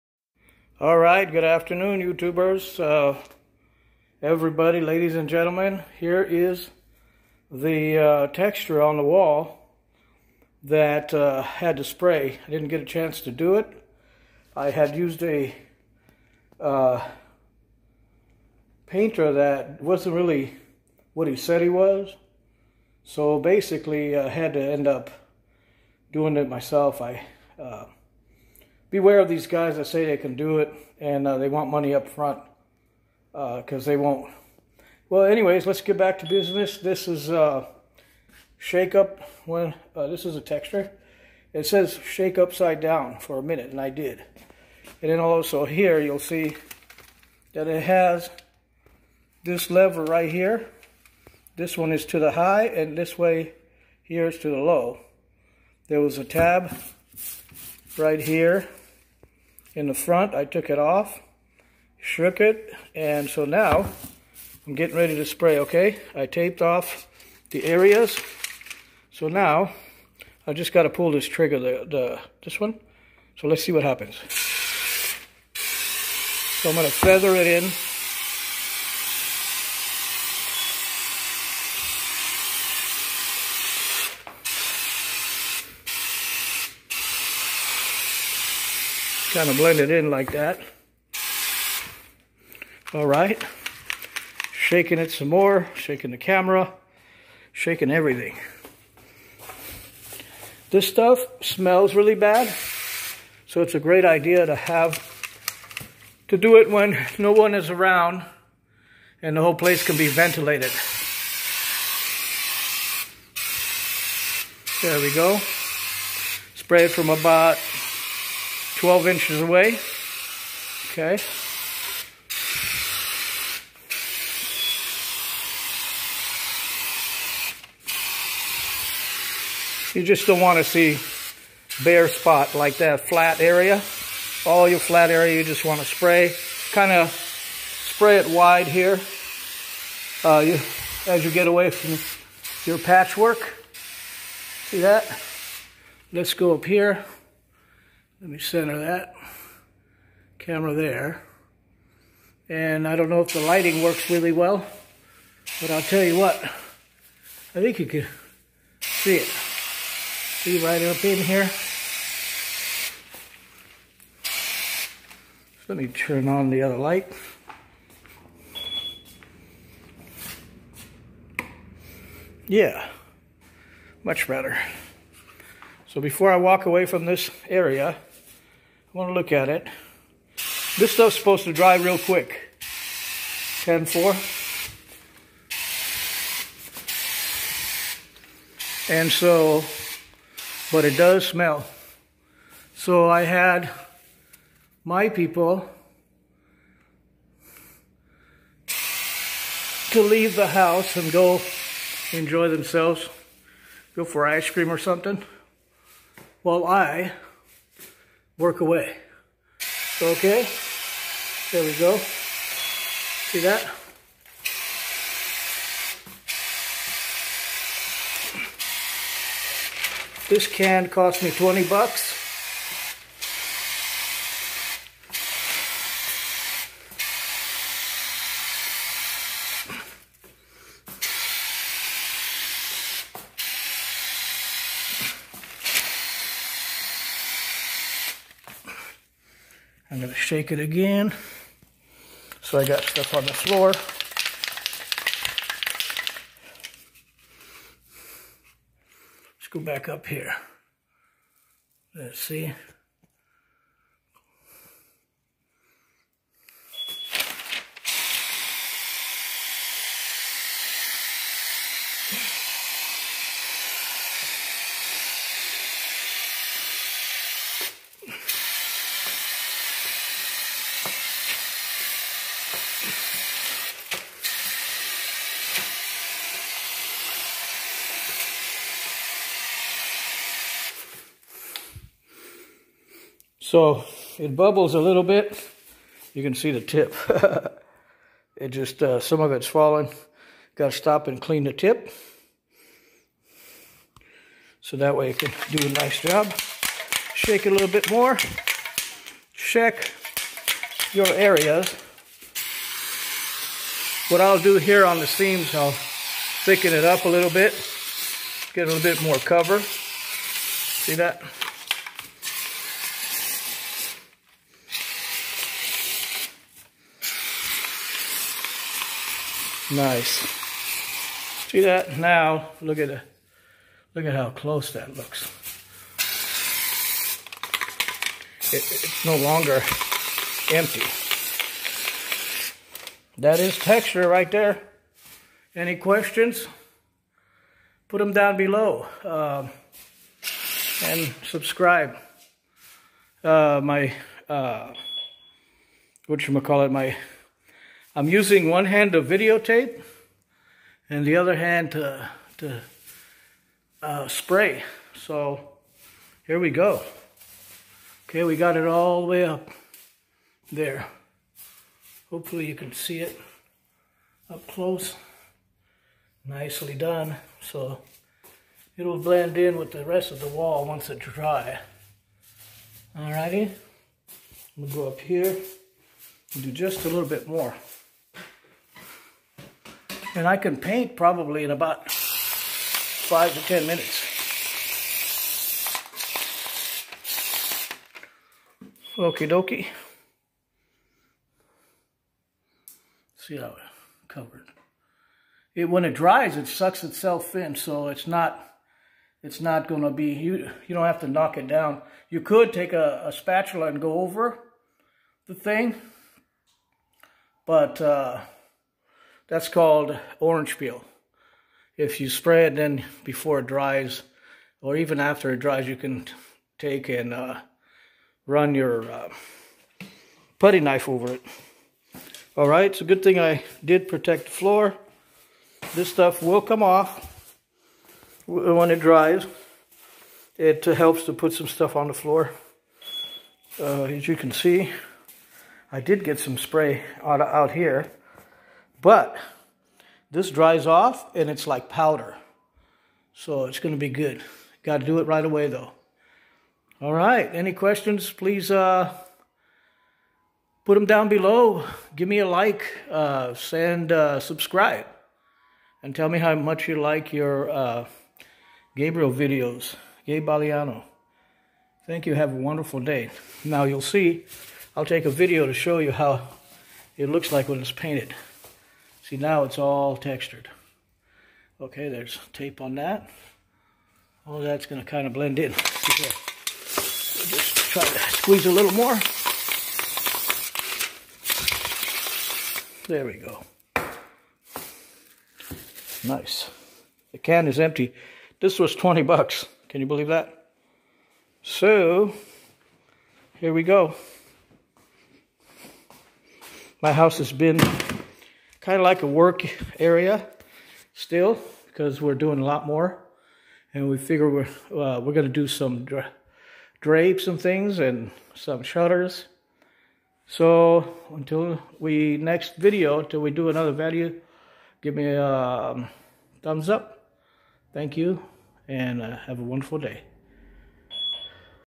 How to spray wall texture from a can the right way
Spraying wall texture from a can